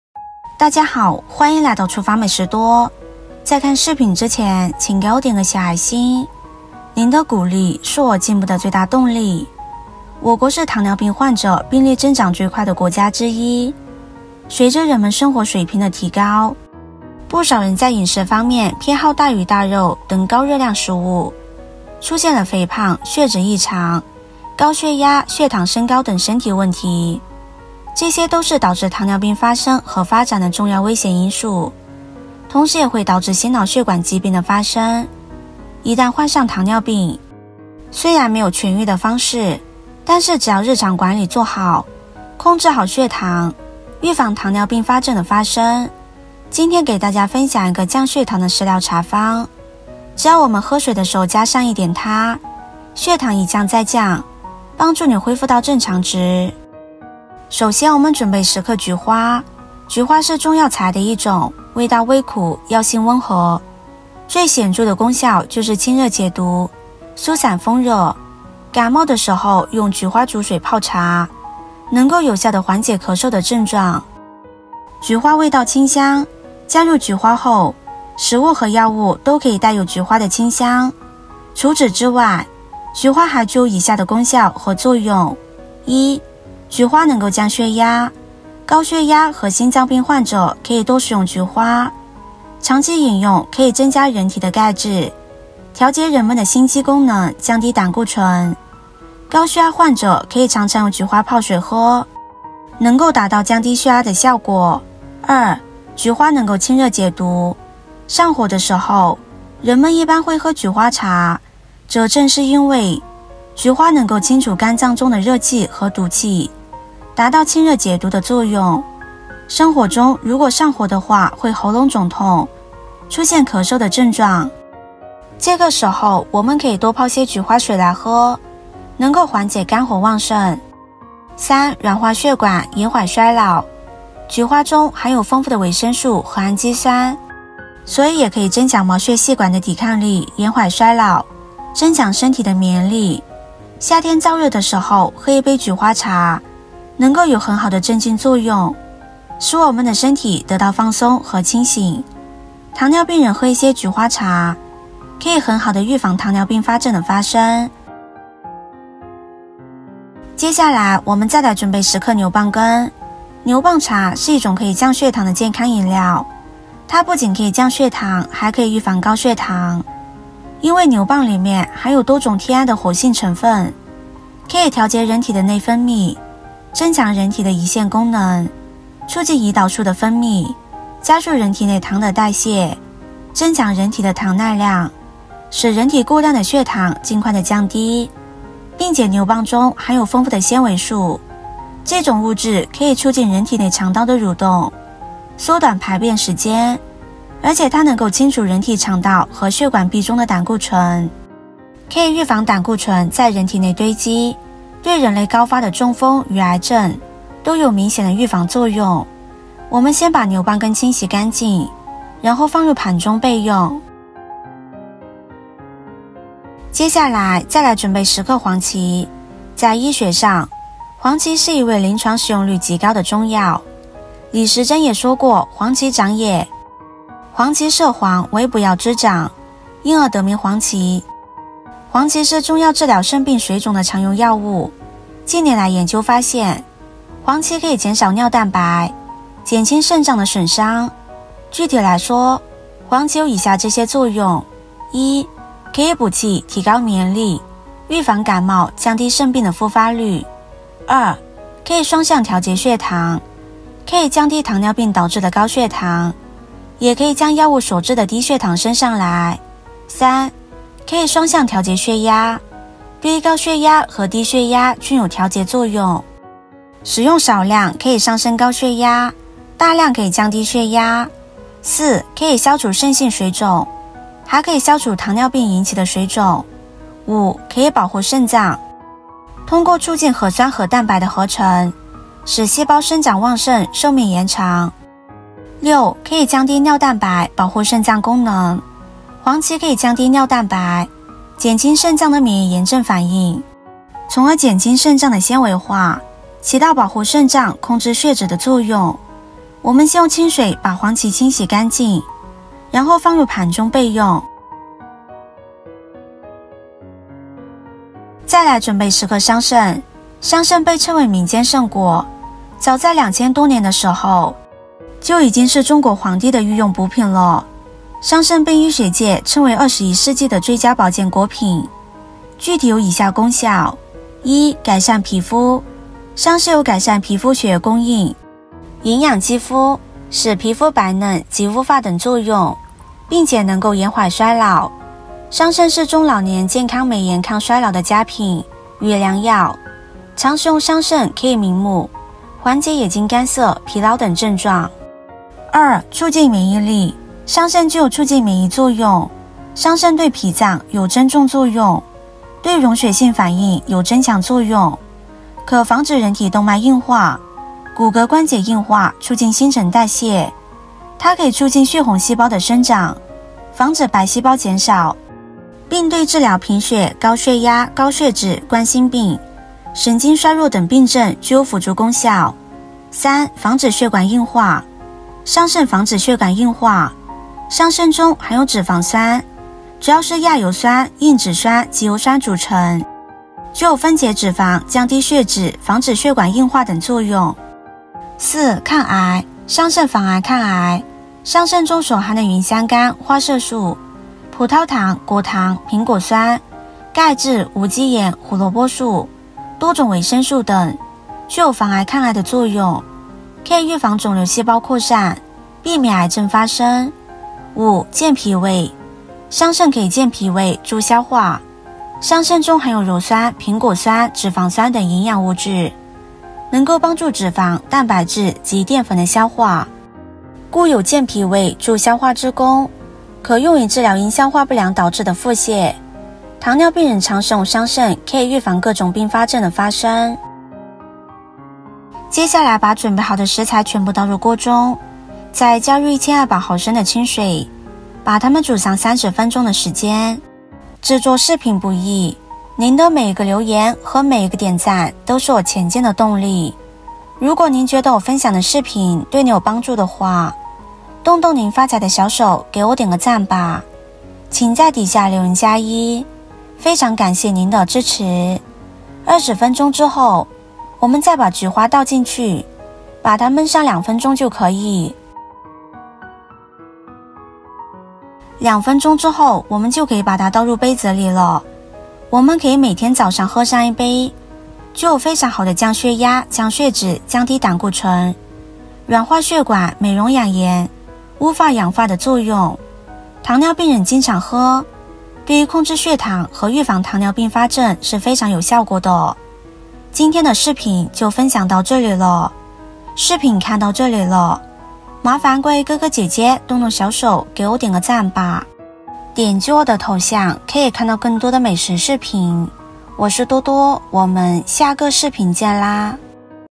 早上喝水時加點它，血糖一降在降，喝一次就恢復到正常值，從此結束了20多年的糖尿病，血脂血壓也正常了
Add it when you drink water in the morning, once the blood sugar drops, it will return to normal after one drink.

Since then, I have ended more than 20 years of diabetes, and my blood lipids and blood pressure are also normal.